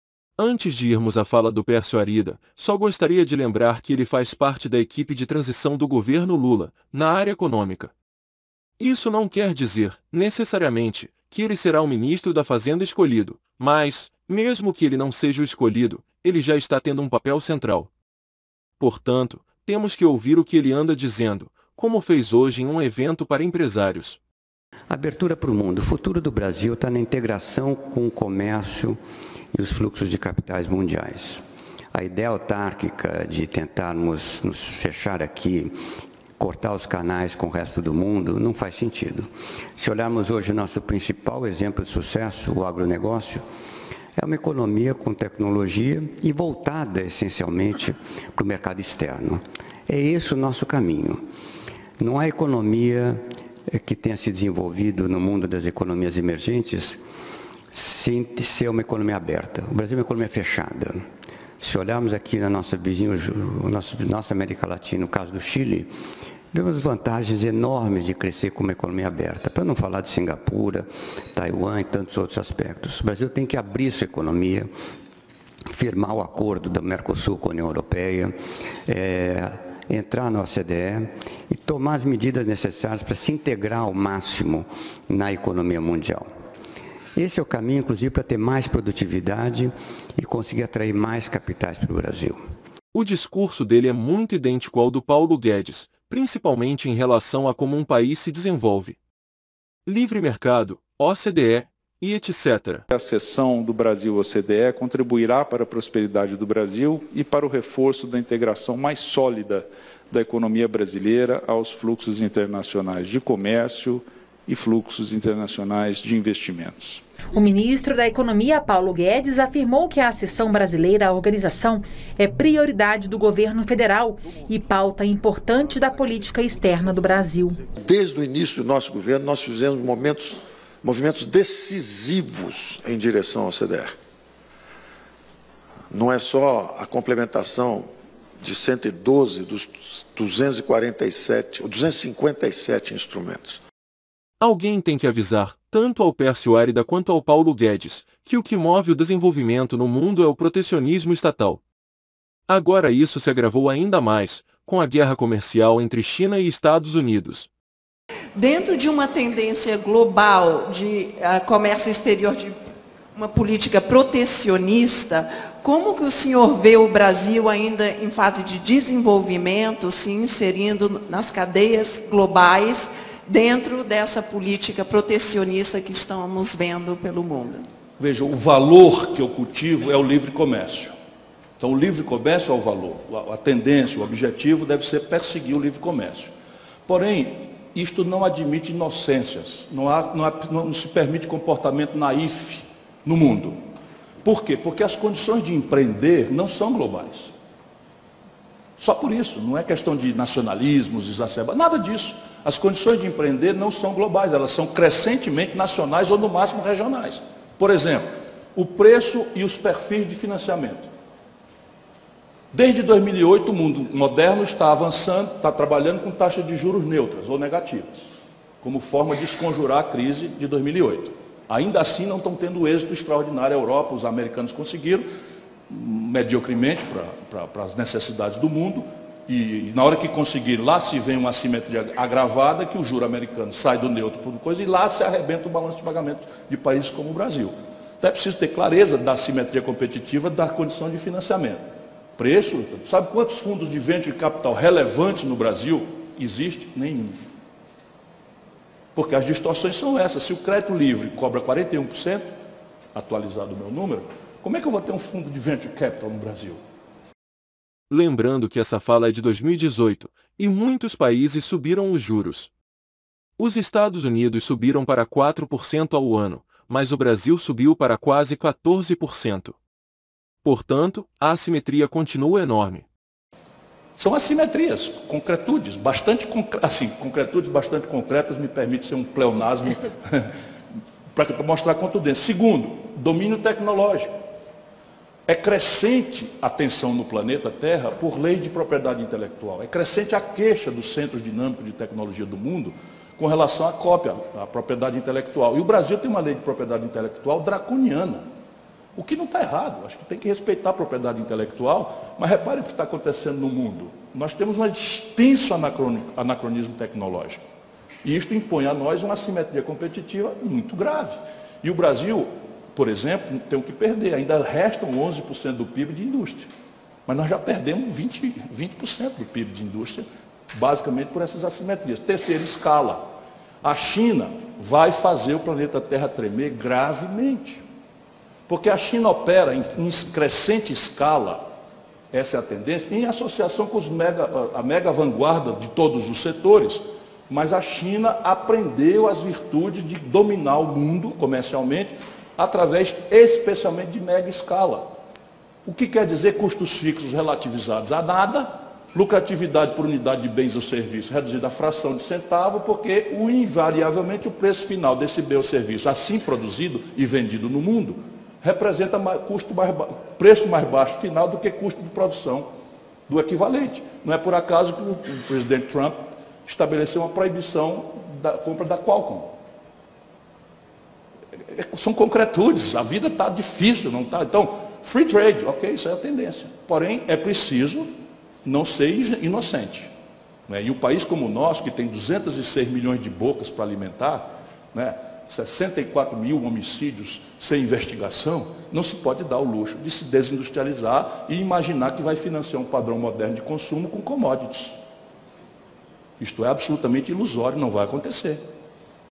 Ciro Gomes diz erros de Pérsio Arida que é igual ao Guedes e que pode ser ministro do Lula
Neste vídeo, é falado sobre o que o Pérsio Arida disse hoje, 15/11, que é semelhante ao discurso do Paulo Guedes, Lula vai ser mais liberal?, Ciro Gomes explica os erros nas análises desses liberais em relação ao livre comércio no mundo